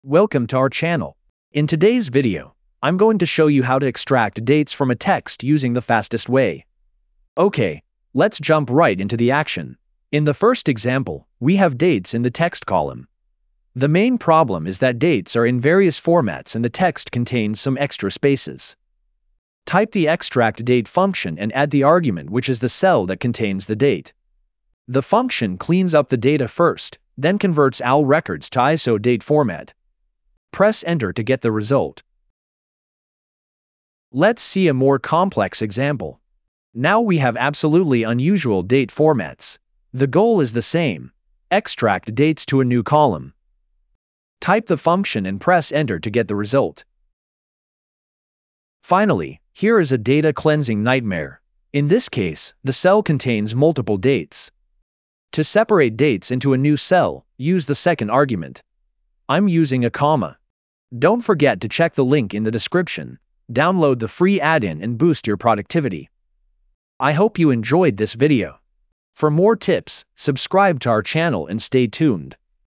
How to Extract Dates from Text in Excel - Formula to Get Date from cell
In today's video, I'm going to show you how to extract dates from a text using the fastest way. Okay, let's jump right into the action. In the first example, we have dates in the text column. The main problem is that dates are in various formats and the text contains some extra spaces. Type the EXTRACTDATE function and add the argument which is the cell that contains the date. The function cleans up the data first, then converts all records to ISO date format. Press enter to get the result.

Let us see a more complex example. Now we have absolutely unusual date formats. The goal is the same: extract dates to a new column. Type the function and press enter to get the result.

Finally, here is a data-cleansing nightmare. In this case, the cell contains multiple dates. To separate dates into a new cell, use the second argument. I'm using a comma.

Chapters:
00:00 Intro
00:10 How to extract dates from a text?
00:34 Complex Data Cleaning Example
00:49 Extract multiple dates from a text in Excel